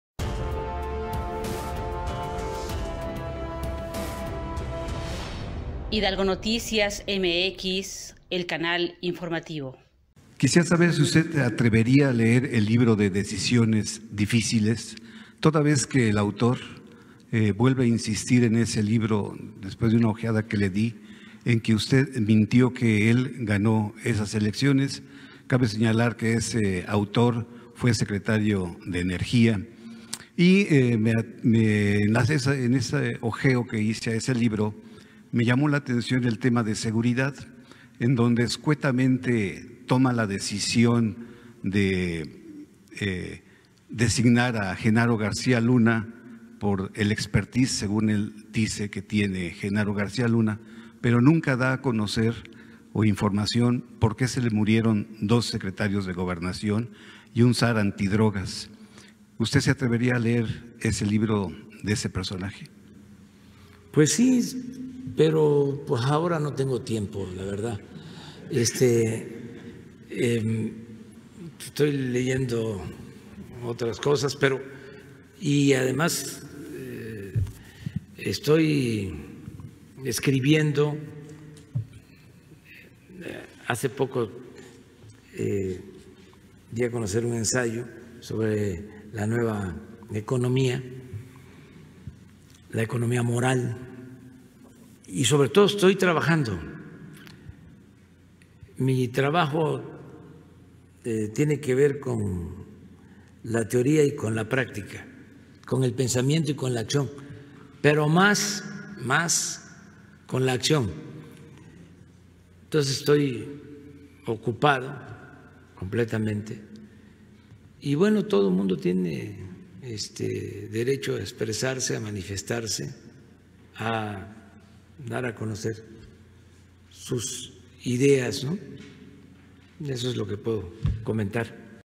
Lord Molécula a AMLO: Usted se atrevería a leer el libro de Decisiones Difíciles de Felipe Calderón?
AMLO le respondió a Lord Molécula: Pues sí, pero ahora "no tengo tiempo", la verdad.